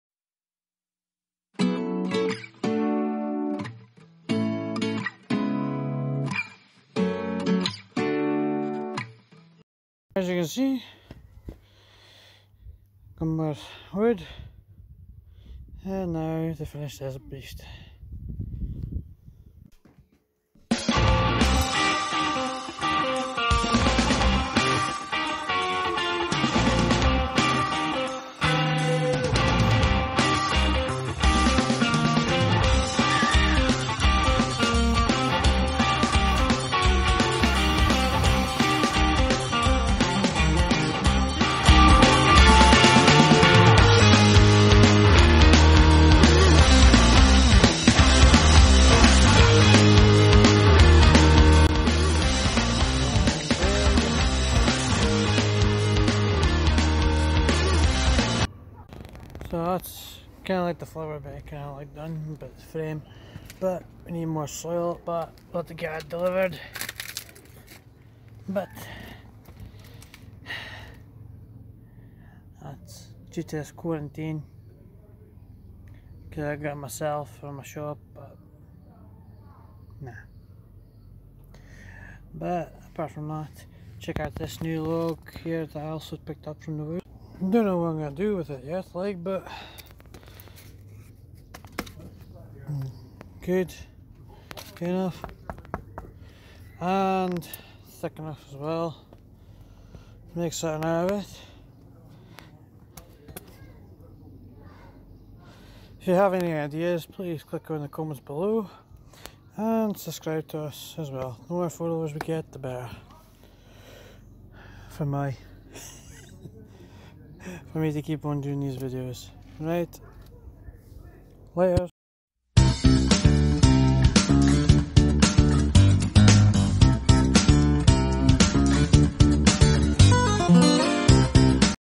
Triple Aces artist LAC second day on flower bed.
Had to get more wood from woods at 6am omg.